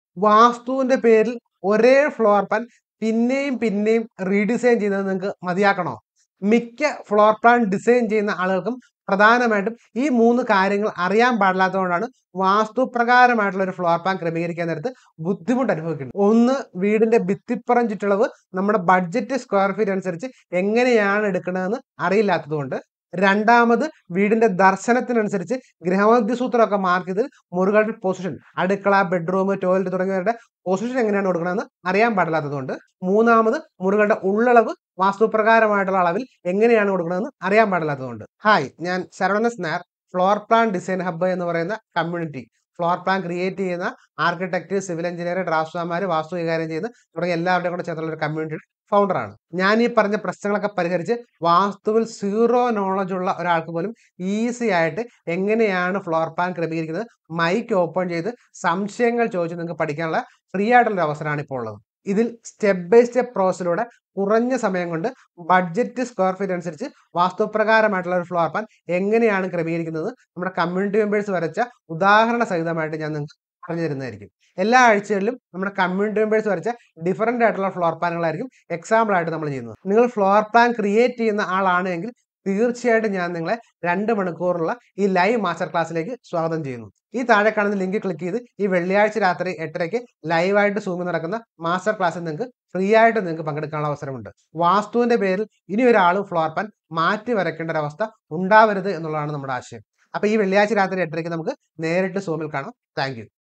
Floor Plan Design Malayalam: Free Live Class on Kerala's First Vastu & Space Optimization
Tired of Redesigning Your Floor Plans Over and Over Due to Vasthu Issues?

Most floor plan designers struggle with Vasthu compliance because they lack knowledge in these three key areas:

1️⃣ Outer Brick Wall Selection: They don't know how to take outer wall selections according to their budget square feet.
2️⃣ Room Positioning: They are unaware of how to position rooms
3️⃣ Room Inside Dimensions: They don't know how to adjust room inside dimensions according to Vasthu guidelines.

Are you looking to design the perfect floor plan for your dream home in Kerala? Look no further! Join us for a Free Live Interactive Master Class where we'll dive into the world of Vastu & Space Optimized Floor Plan Design. This is your chance to learn from Kerala's leading expert in floor plan design, Saravanan S Nair.

🔹 What You'll Learn:

Essential principles of Vastu for home design.
Step-by-step process to create space-efficient floor plans.
Tips and tricks for modern and traditional Kerala house designs.
Practical examples and case studies.
Interactive Q&A session to address your specific queries.
🔹 Why Join?

Learn in Malayalam, ensuring a better understanding.
Get expert insights from Saravanan S Nair, founder of the Floor Plan Design Hub.
Perfect for homeowners, architects, and anyone interested in Vastu and home design.
🔹 Event Details:

Date: FRIDAY
Time: 8.30 TO 10.30 PM
Platform: ZOOM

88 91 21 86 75

Don't miss this opportunity to transform your home design journey with expert guidance and practical knowledge.